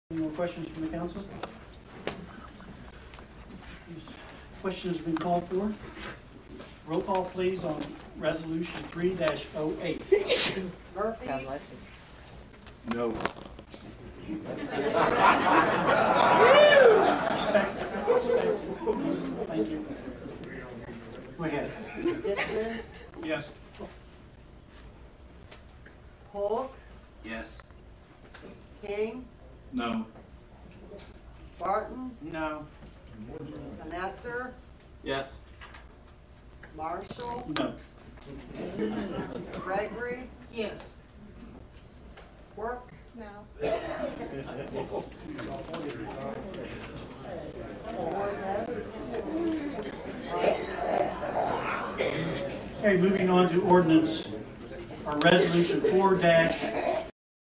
Voting the Code of Ethics DOWN, Feb 4th, 2008
Here is our illustrious city council voting NO to adopting a Code of Ethical Behavior...WOW!

I have to give Jerry Dishman credit for voring "Yes", good for him!